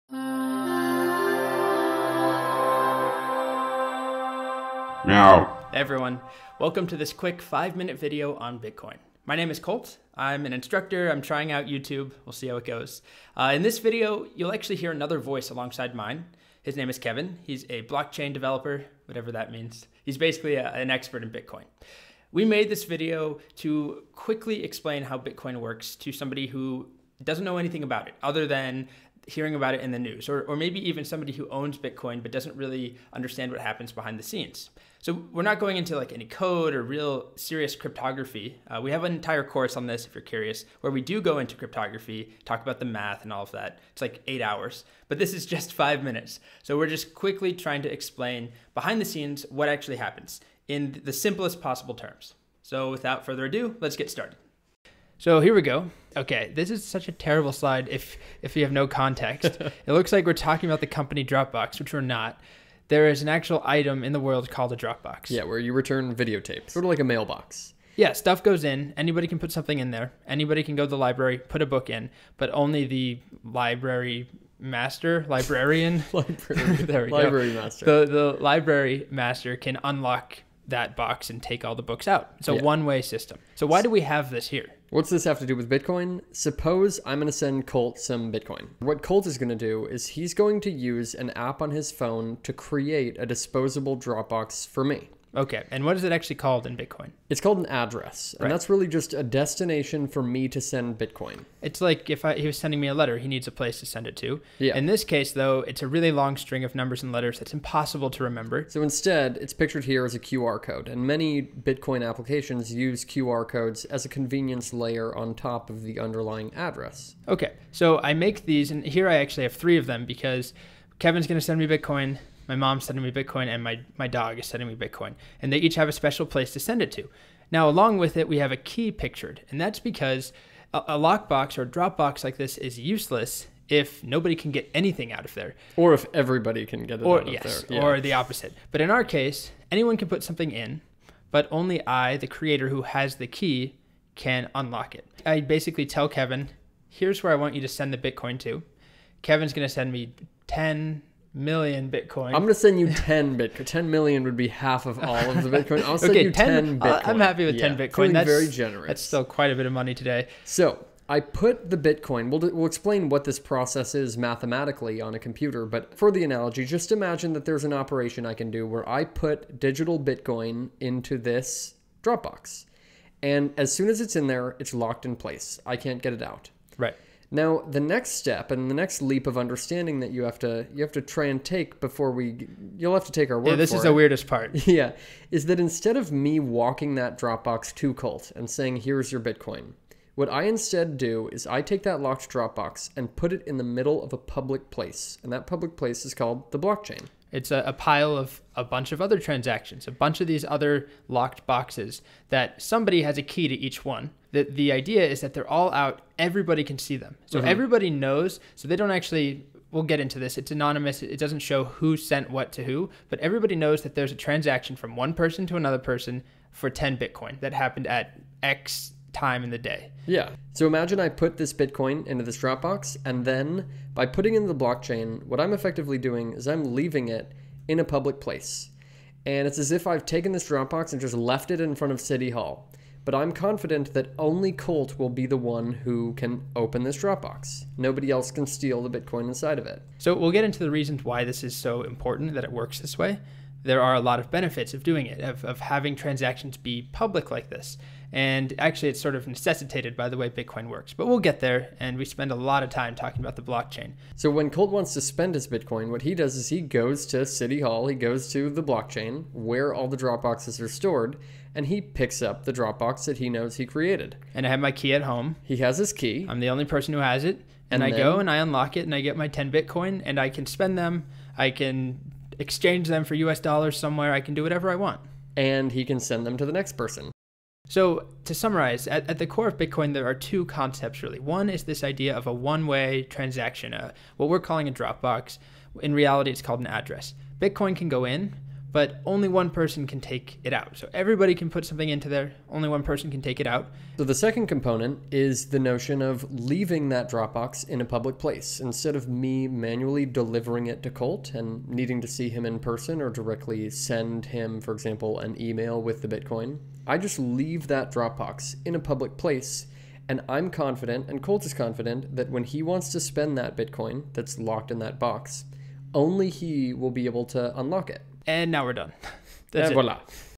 Bitcoin Transaction Explained in 5 Minutes
In this quick video, we do our best to explain the mechanics of a single Bitcoin transaction in 5 minutes or less.  We explain things like addresses, wallet apps, private keys, and the blockchain.

If you're ready to launch your career as a software engineer, check out my job-guaranteed bootcamp with Springboard